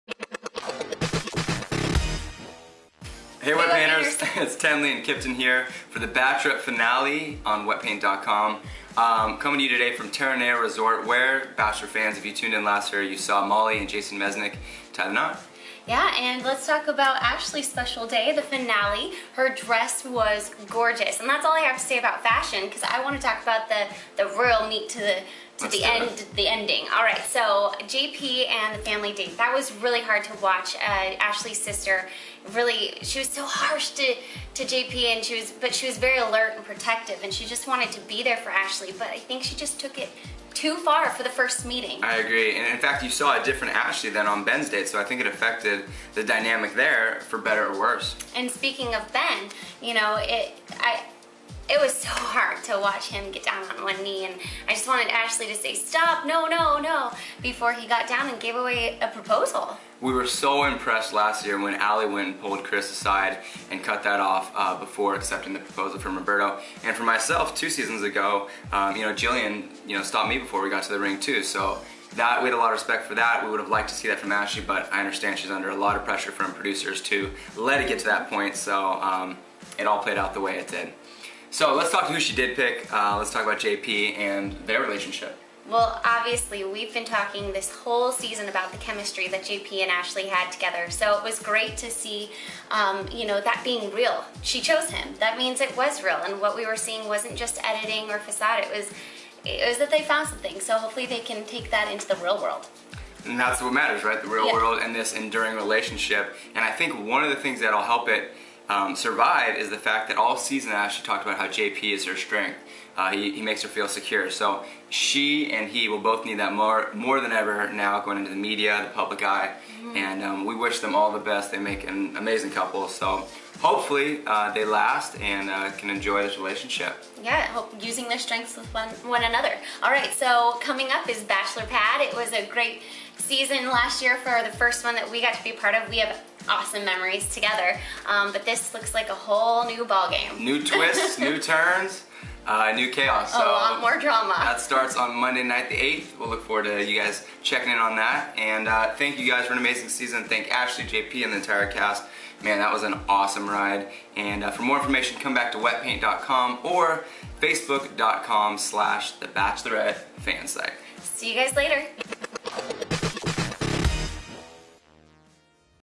Tenley & Kiptyn Reveal Why Ashley Didn't Stop Ben From Proposing in the Bachelorette Season 7 Finale
- Visit us for more Bachelorette exclusives, gossip, and spoilers!

Past Bachelorettes Ali Fedotowsky and Jillian Harris put the kibosh on finale proposals they didn't plan to accept. So why didn't Ashley Hebert? Our pals and Bachelor Pad alums Tenley and Kiptyn speculate why we witnessed this SUPER AWKWARD CRINGE-WORTHY moment. But this Team Cupcake duo isn't holding anything against the dancing dentist, providing some words of wisdom for maintaining a post-Bachelor love! Listen up, Ash — they know of what they speak...

Tune in next week for Kip-Ten's next Wetpaint vlog!